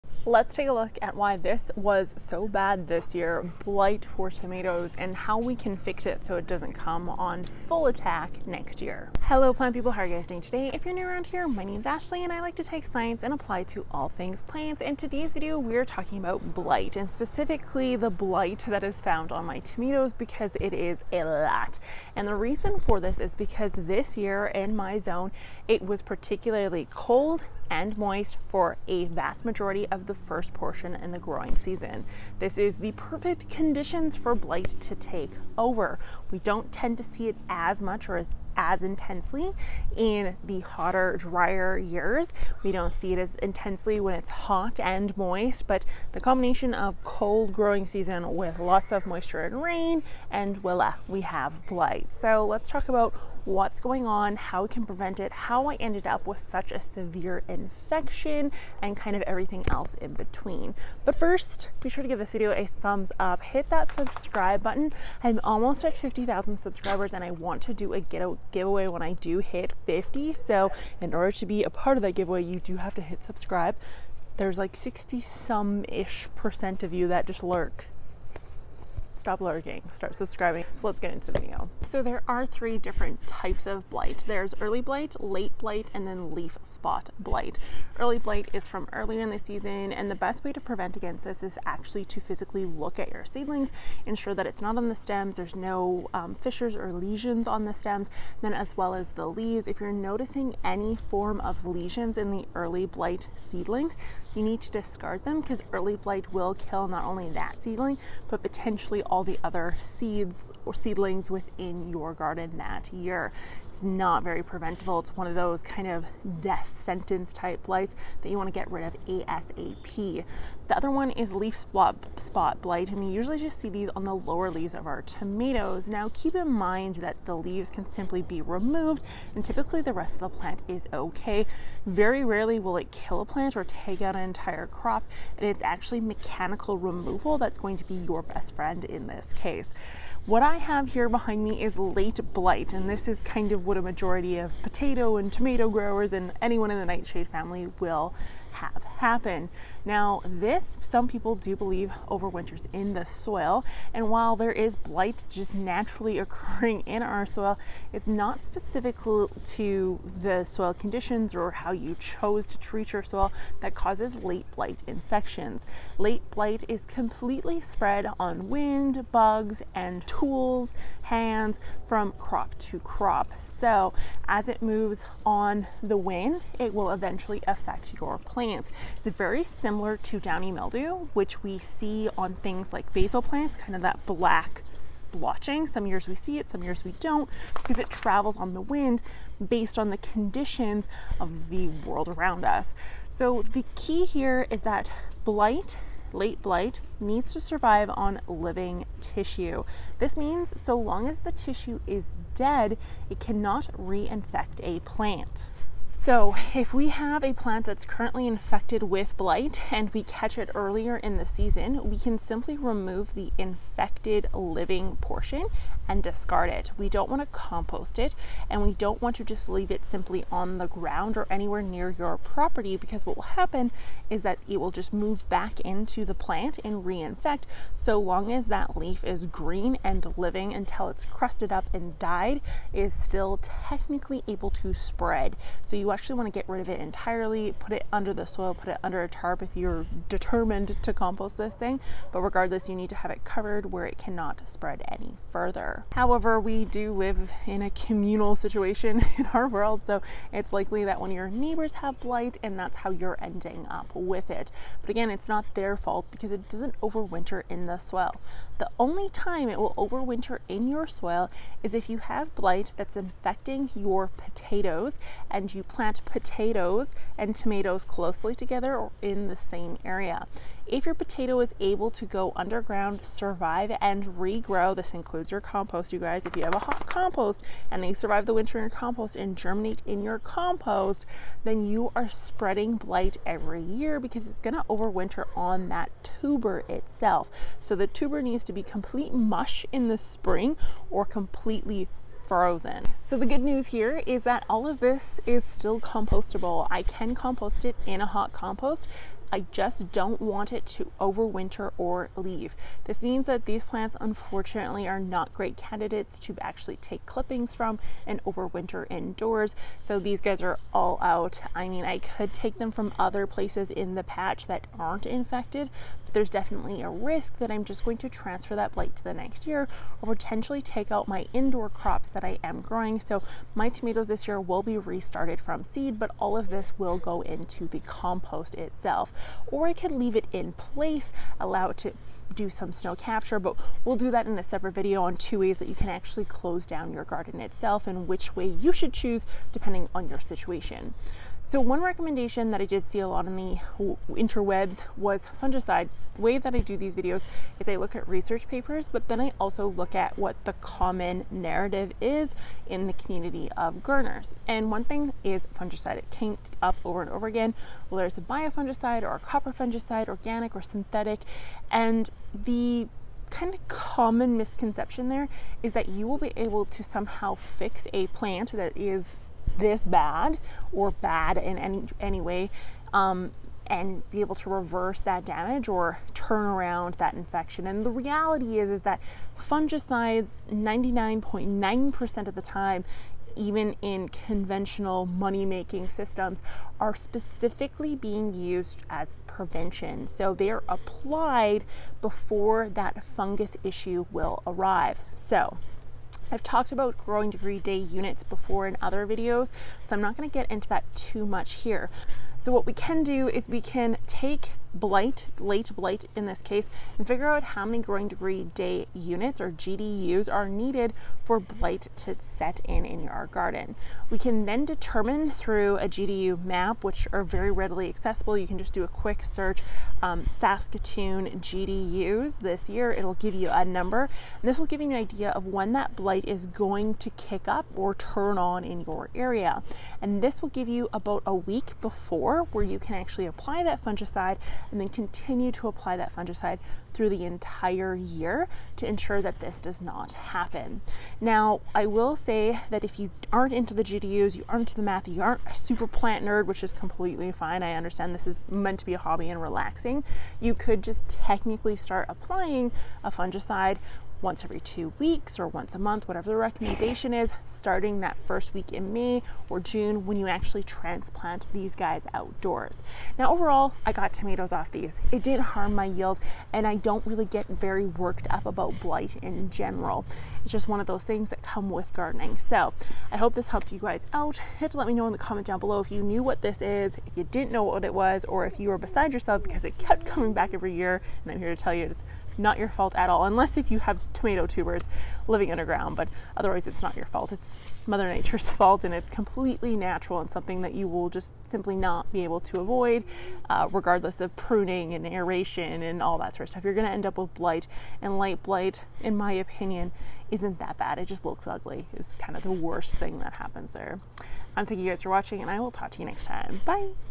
Does Blight Over Winter In Your Garden Soil? How To Control Blight In Your Garden | Gardening
Tomato blight can destroy a crop if not managed properly. This gardening in Canada video looks at tomato blight and how to control it in your garden.

Paperback Houseplant Planner:
Paperback Garden Planner:
Digital Download Planners

Join our monthly newsletter! newsletter.gardeningincanada.net